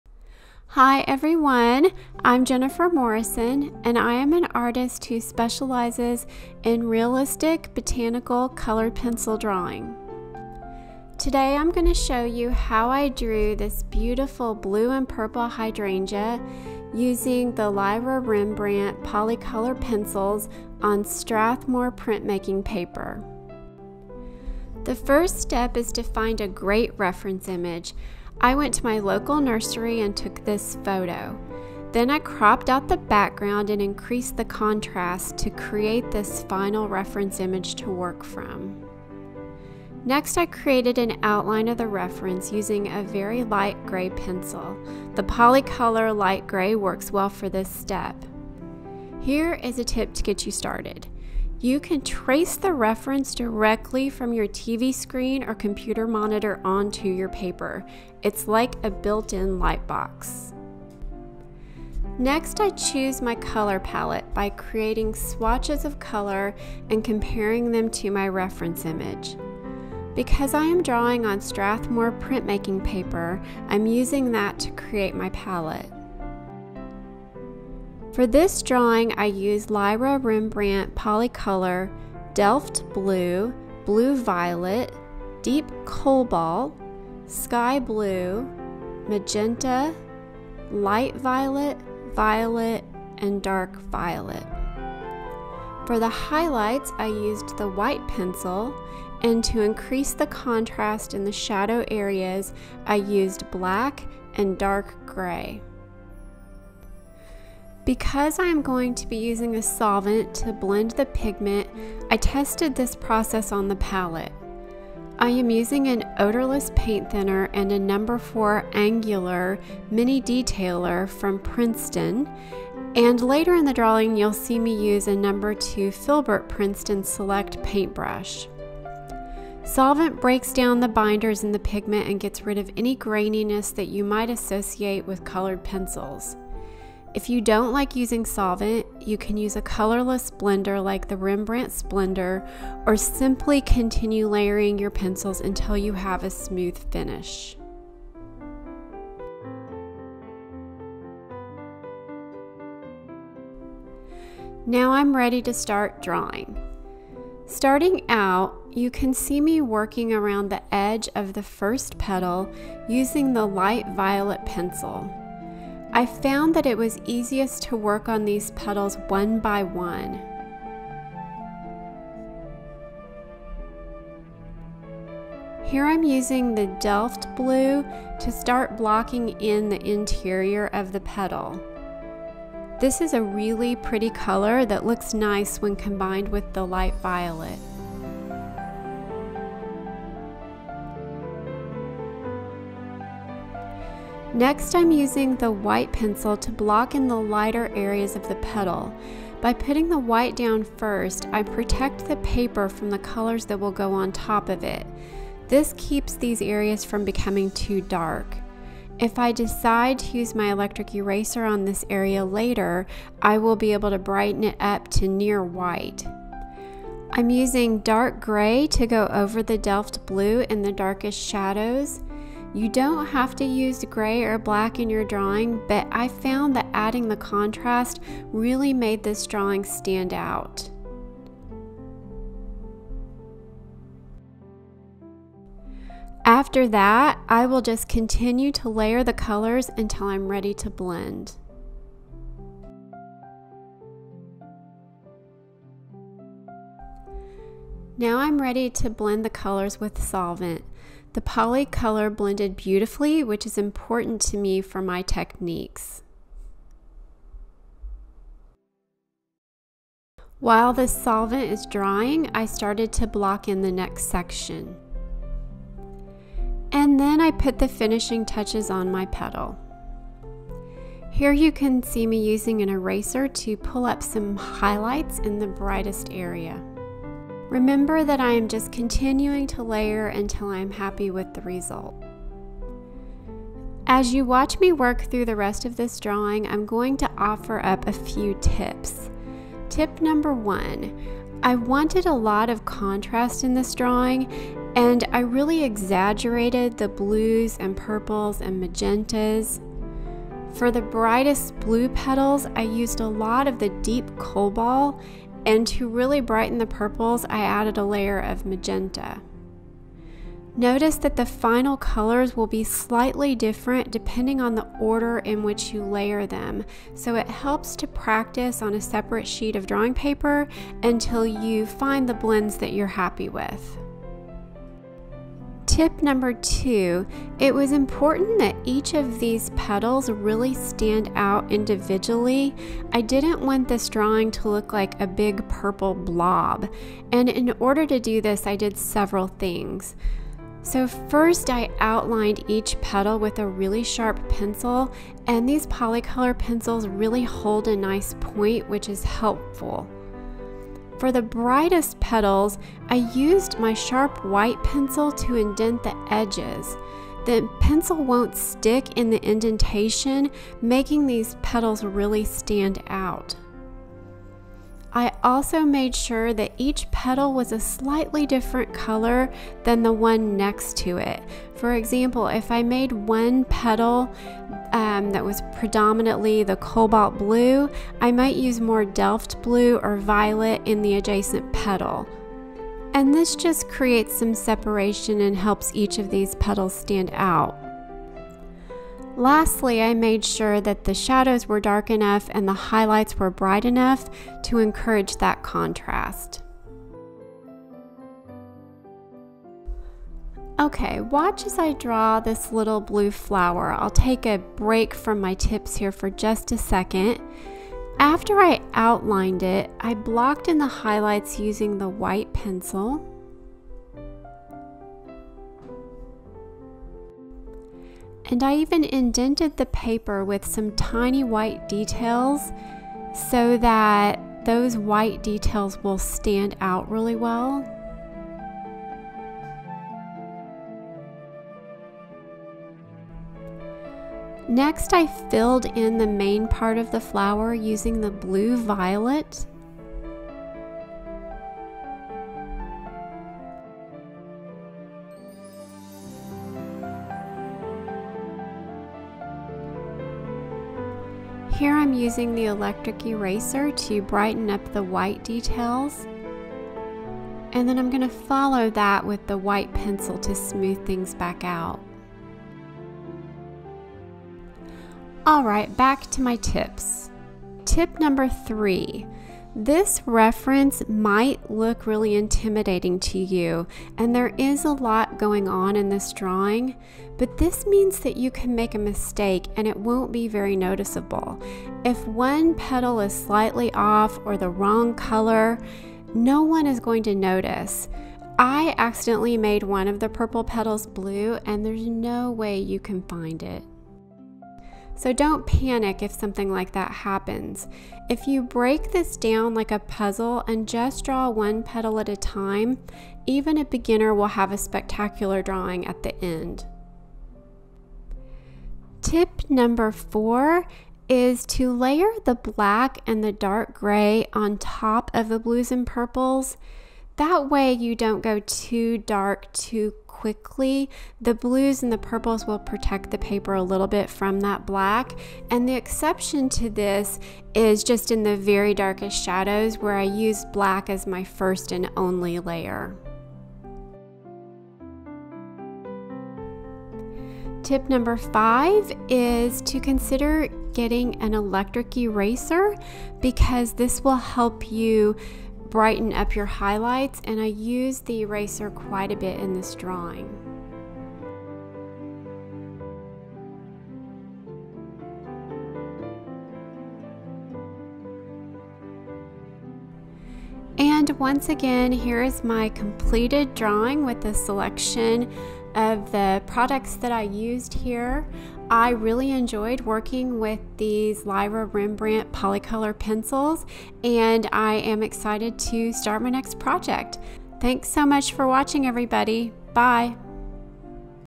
Colored Pencil Hydrangea Tutorial
Follow along with Jennifer Morrison as she demonstrates how to draw a stunning hyper-realistic hydrangea in colored pencil.

Materials:
► Strathmore 400 Series Printmaking paper (280gsm, medium surface)

► Lyra Rembradnt Polycolor Pencils in:
- Delft Blue
- Violet
- Light Violet
- Deep Cobalt
- Blue Violet
- Dark Violet
- Sky Blue
- Magenta
- White
- Dark Gray

► Blending Solvent

► Mini eraser & electric eraser